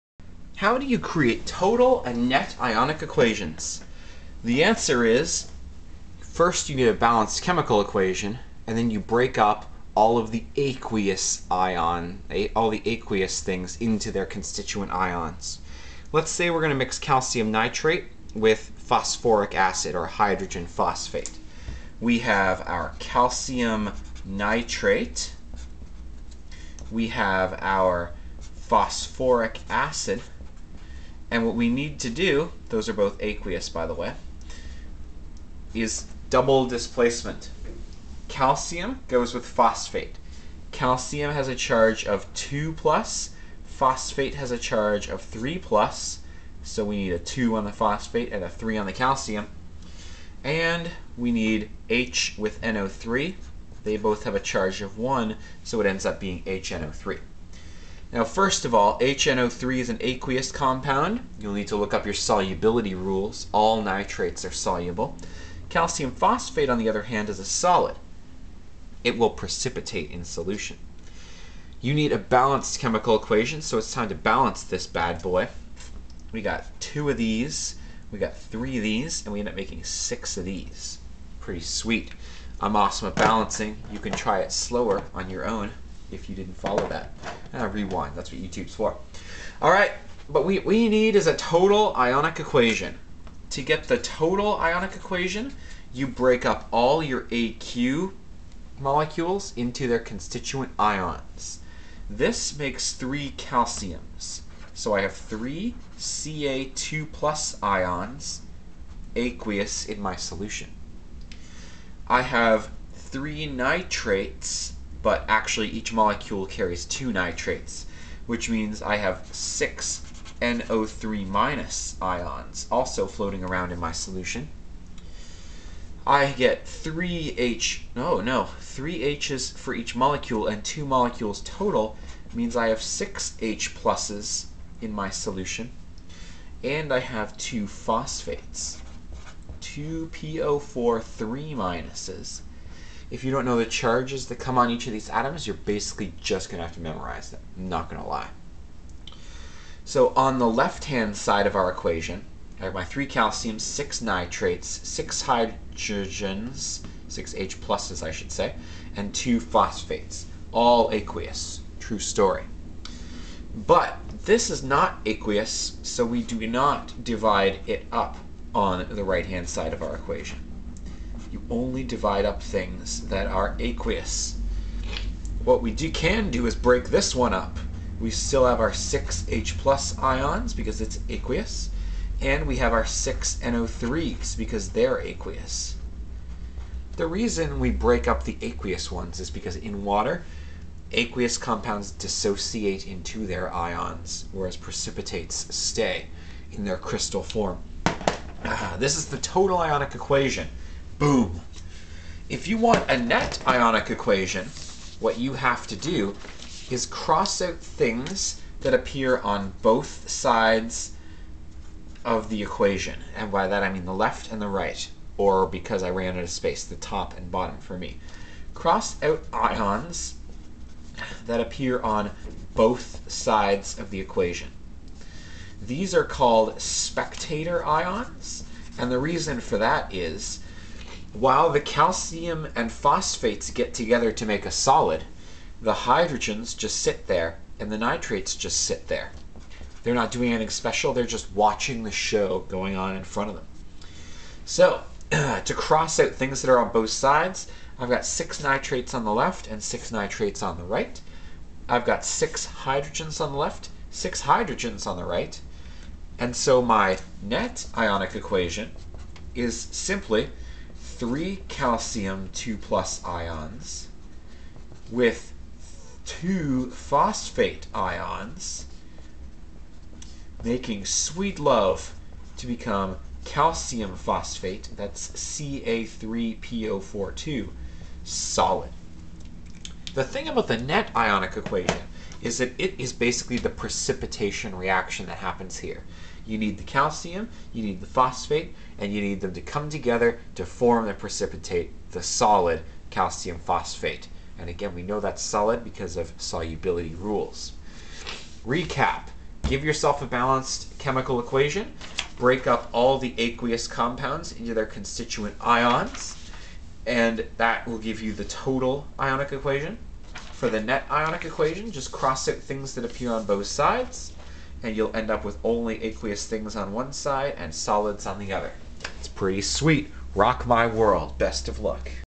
How to Write Total and Net Ionic Equations (Easy)
How to write total and net ionic equations.
1. Write a balanced chemical equation
2. Break up all the (aq) compounds into its ions (this is TOTAL)
3. Get rid of spectator ions (things that appear on both sides) (this is NET)